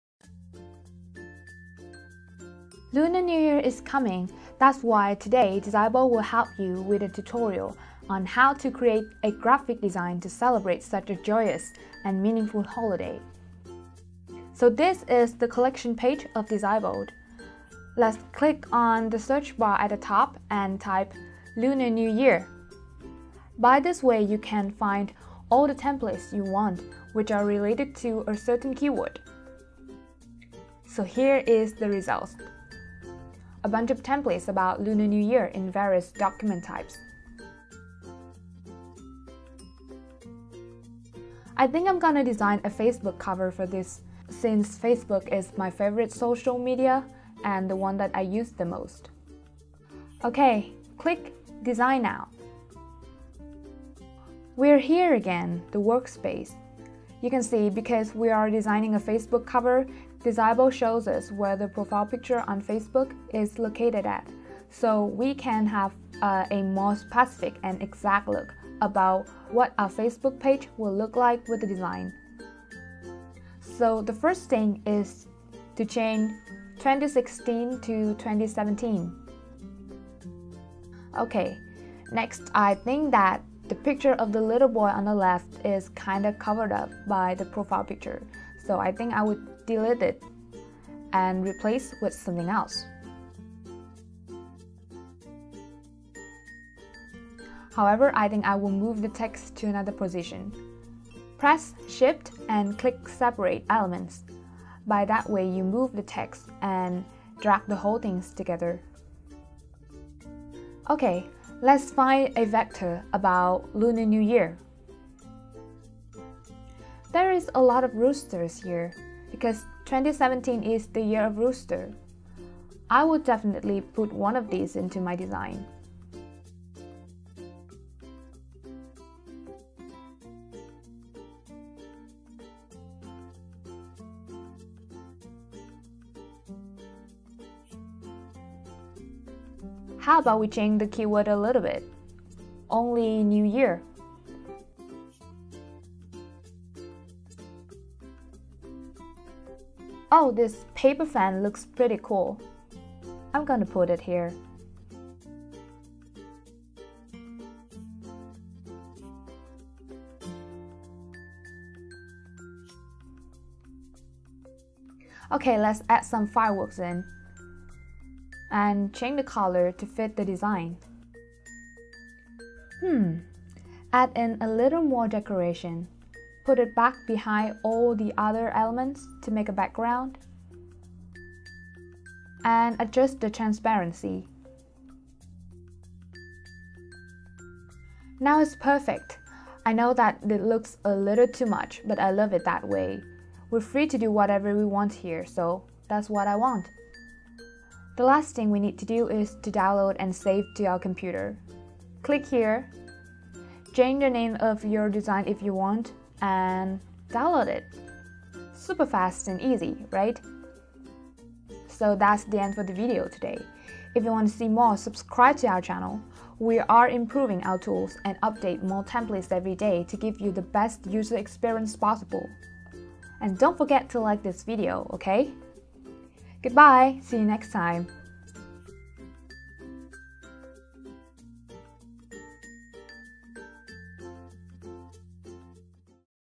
How to design Lunar New Year Facebook Cover?
a free Online Graphic Design Tool